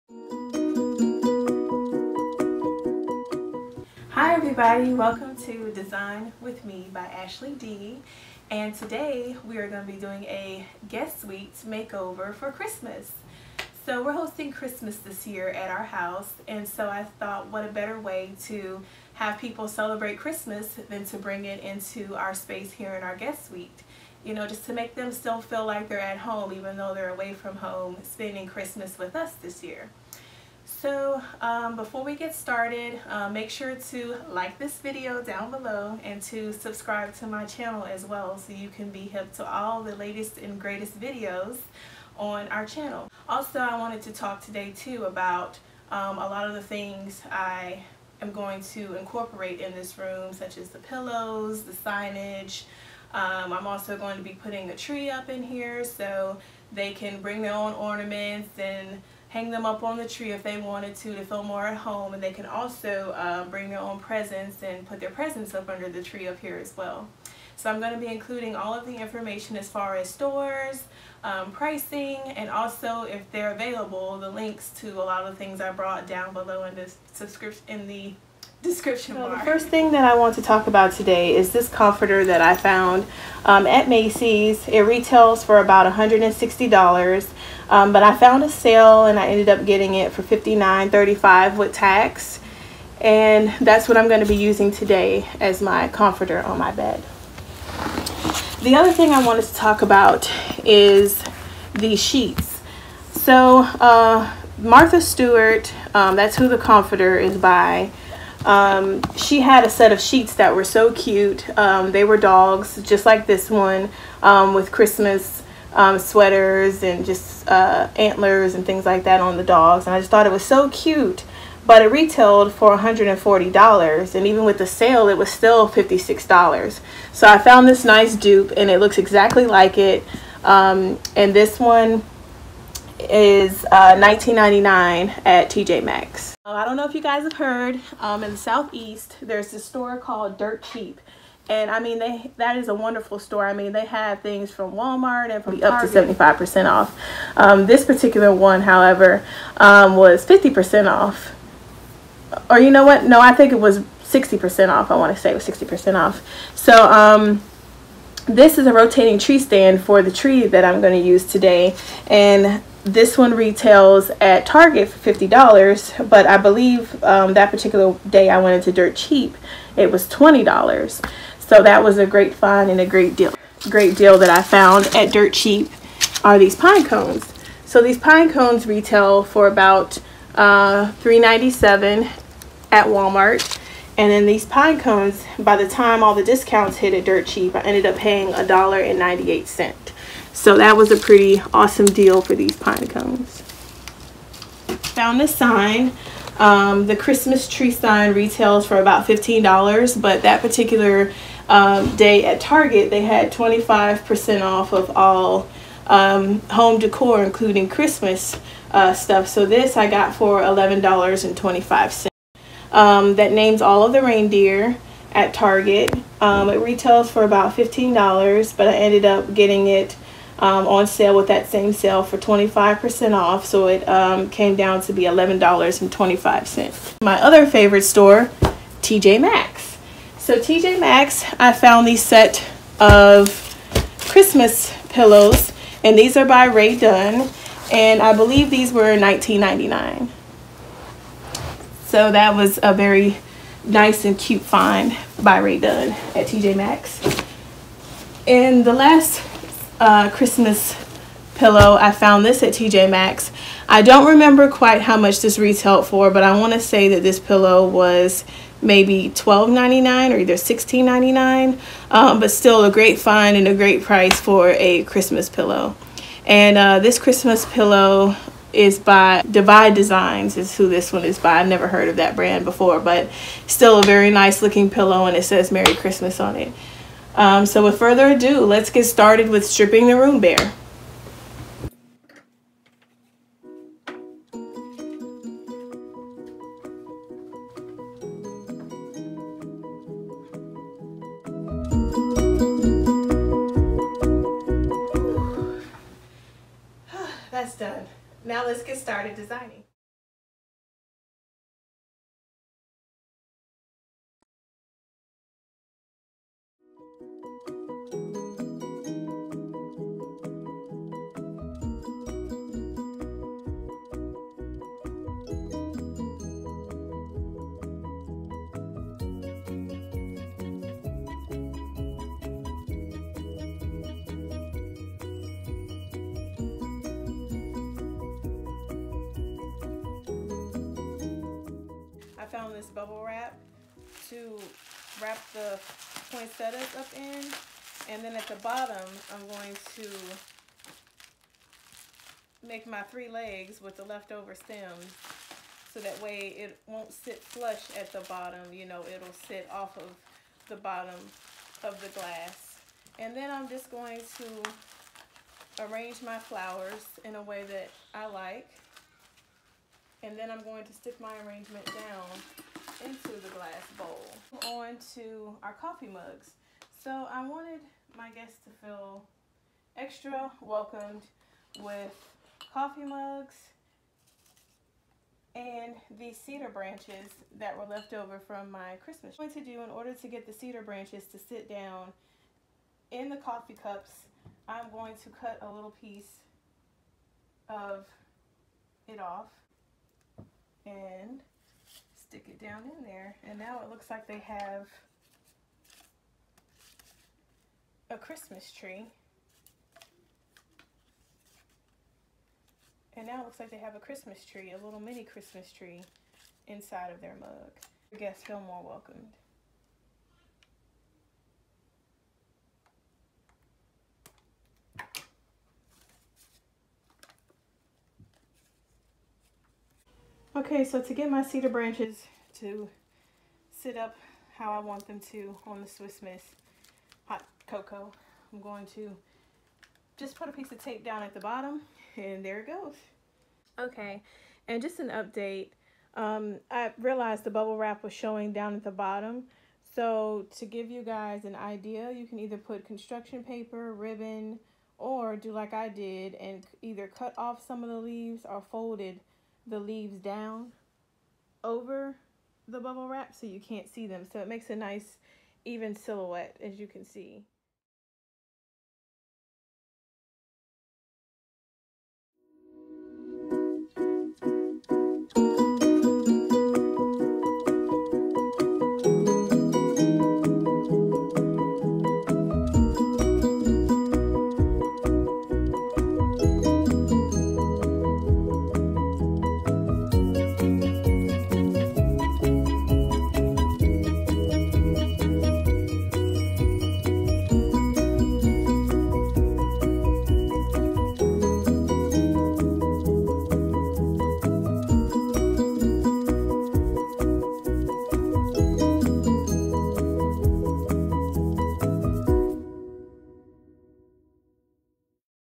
Christmas Guest Suite Makeover
Decorate your guest room with mee Ashley Dee for Christmas. Having guest over and need idea's for decoration? No problem. I have you covered. List of items, pricing and links below (if available).
Macys.com
*Duvet Cover
Martha Stewart Collection

*Sheets
Martha Stewart Collection
Printed Cotton Flannel 3-Pc. $140 brought the dupe by Cynthia Rowley on sale at TJmaxx $19.99.

Dirt Cheap
$20 Rotating Tree Stand regular $50 in Target
$2 Pine Cones regular $3.97 in Walmart

Target.com
$15 Christmas Tree Holiday Sign White - Threshold™

$15 Reindeer Names Chalkboard Sign - Threshold™